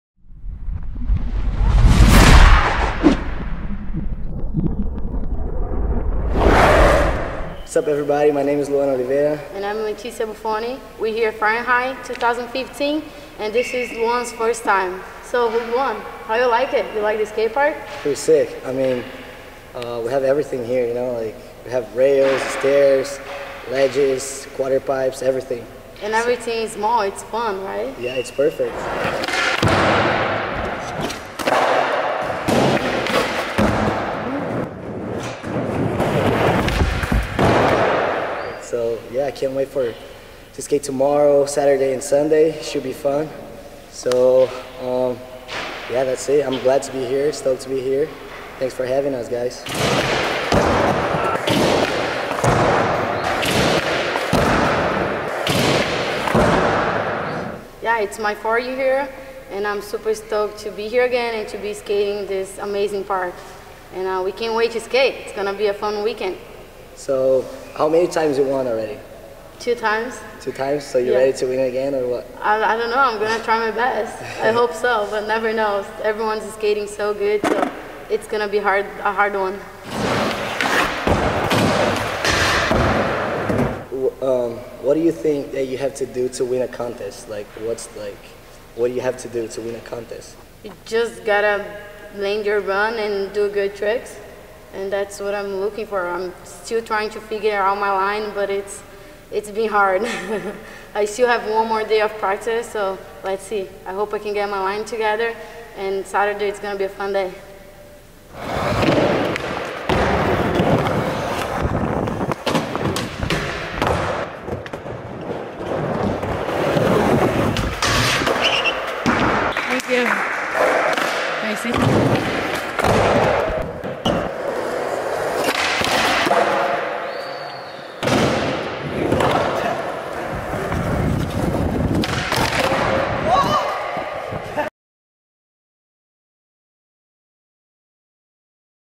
ITW Luan Oliveira & Leticia Bufoni
ITW Luan Oliveira & Leticia Bufoni at Far'n'High 2015
Film and edit Nicolas Jacquemin
Artwork by Tristan Badoual